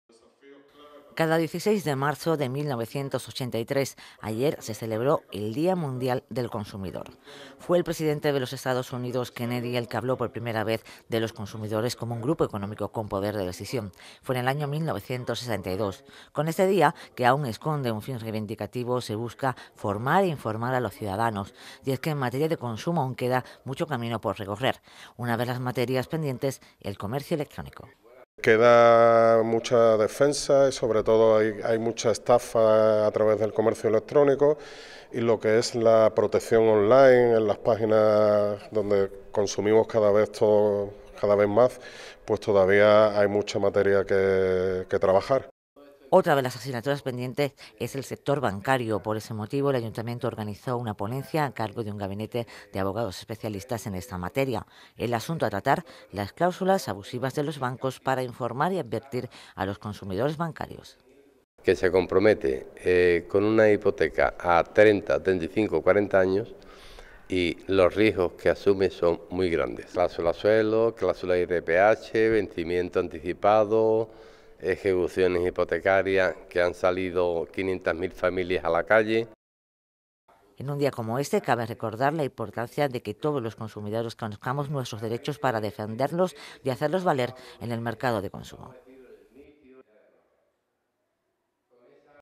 Un Día del Consumidor dedicado a las cláusulas abusivas de los bancos
Como cada 15 de marzo, ayer se celebró el Dia Mundial del Consumidor. Con motivo de esta celebración, aunque se trata también de un día reivindicativo, el Ayuntamiento de Algeciras organizó una ponencia con un único tema a tratar: los derechos de los clientes bancarios ante las cláusulas abusivas.